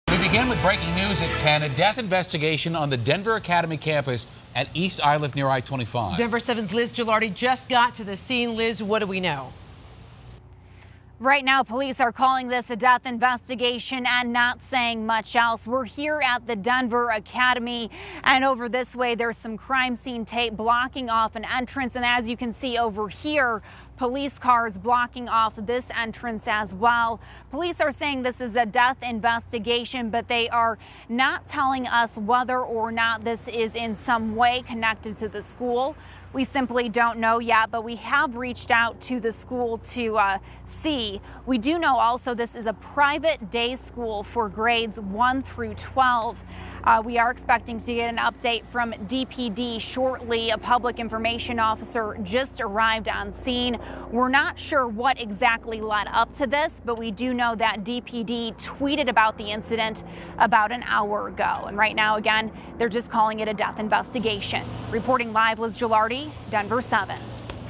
Death investigation underway at Denver Academy
Denver7 reporter Liz Gelardi tells us more about an ongoing death investigation at Denver Academy.